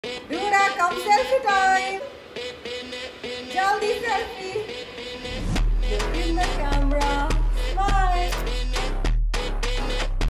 Cap wali selfie 🧢
Vaise to uthte ni h morning me jaldi aaj uth gae to bhi koi fayda ni kyuki end me jaake so hi Jana hota h. Kuch dino me saari insaan wali habits le lenge ye dono. Life ho to aisi